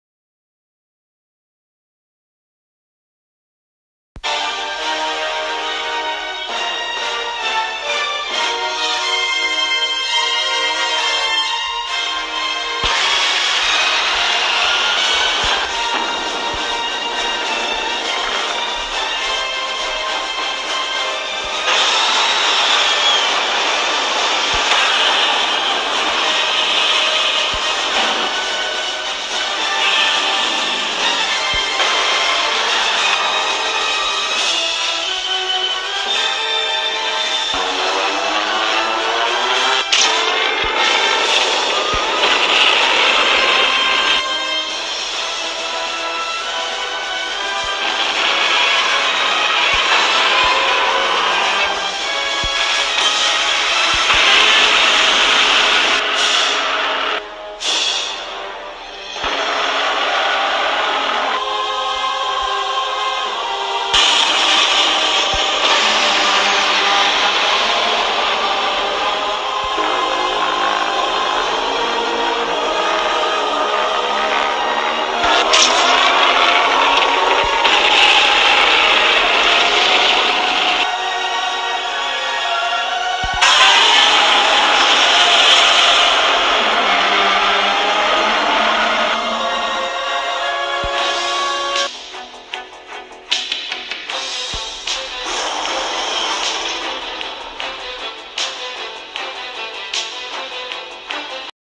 GODZILLA Battles DESTROYAH part 3 Finale
Finally finished and I didn't have to make a part 4, thank you guys for the patience on getting this project done and I can now rest for a while until Godzilla vs Kong.